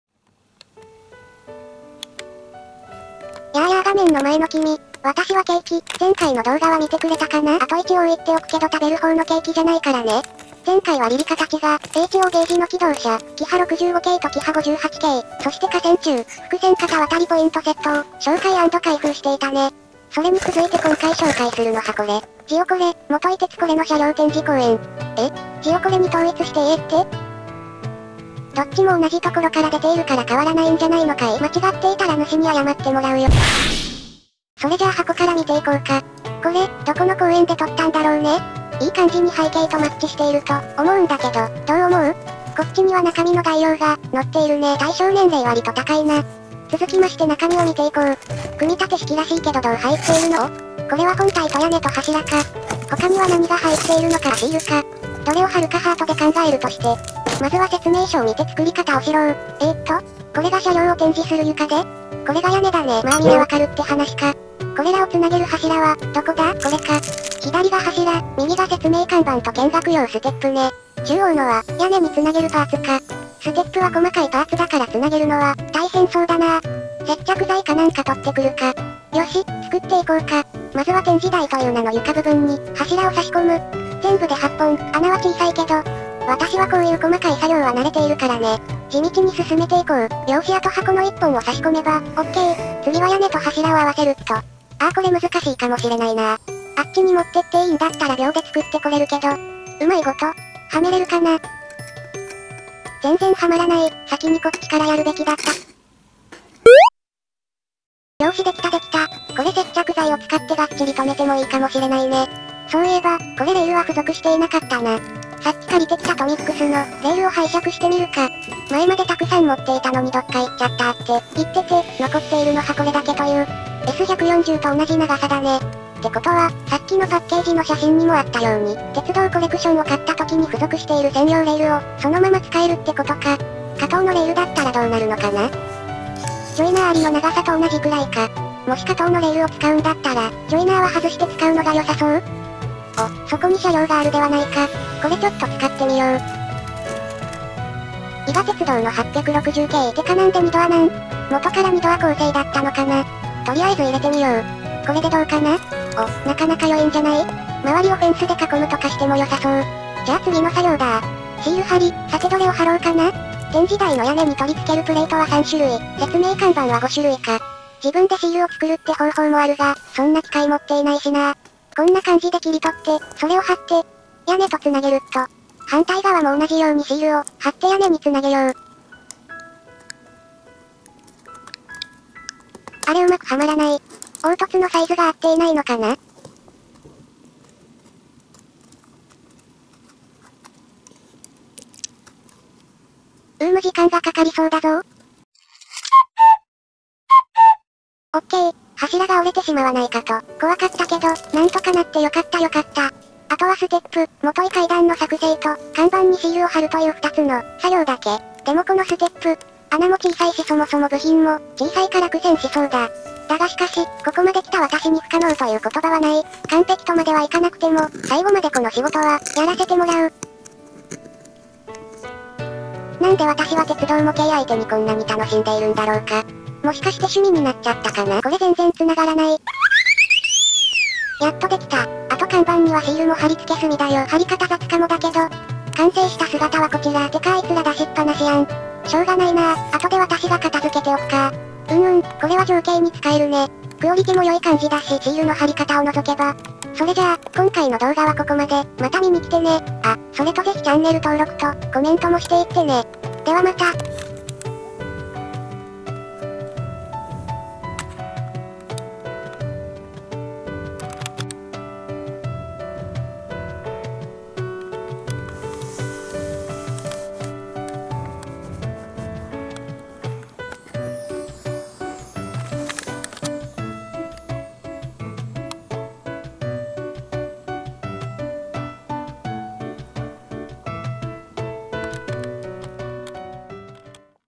トミーテックさん...車両展示公園とか...最高じゃないか!! 組み立てムズかったけど。【東方デフォルメ茶番】【Nゲージ】【ゆっくり実況】【鉄道模型】リリカ達の鉄道模型26.1
前回のリリカの鉄道模型シリーズでちょこっと触れたジオコレ（鉄コレ）の情景コレクションから販売されているとある品を今回は開封&レビュー！車両を展示して静態保存風に演出できる公園施設を組み立てていきます。今回はリリカではなく、埴安神袿姫が担当致します。なぜそうなったかは前回を見てね！

この動画はAviutlとYukkuriUtil（ゆくサク）を使用しております。

音源・曲・効果音・SE等
・ニコニ・コモンズ様 →　commons.nicovideo.jp
・その他etc..